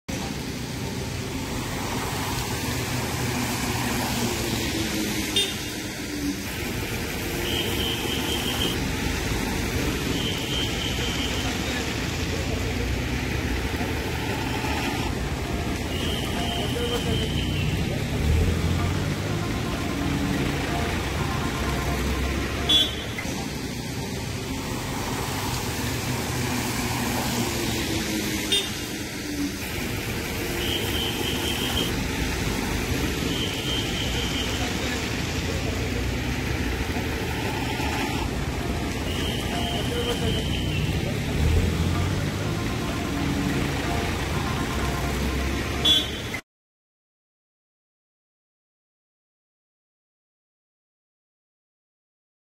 Grenade Attack In Bijbhera Anantnag, Cop Injured
A Cop was injured after militants hurled a grenade towards police party in Padshahi Bagh area of Bijbhera in South Kashmir’s Anantnag district on Wednesday evening.